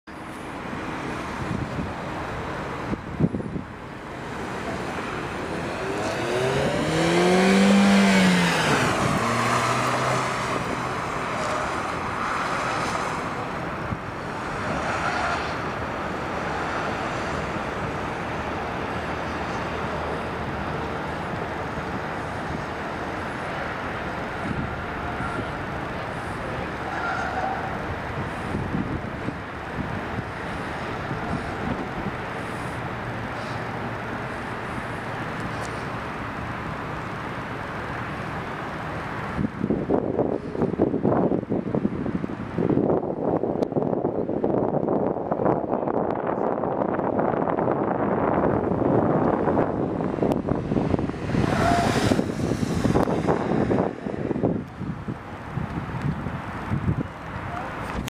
Ford 2014 Fiesta Test Drive (Outside View)
Changes from 2013 to 2014:

-MyKey® programmability, limiting speed and audio levels
-New chrome grille and hood
-Available SYNC® with Myford Touch® 6.5-inch screen, featuring software that includes u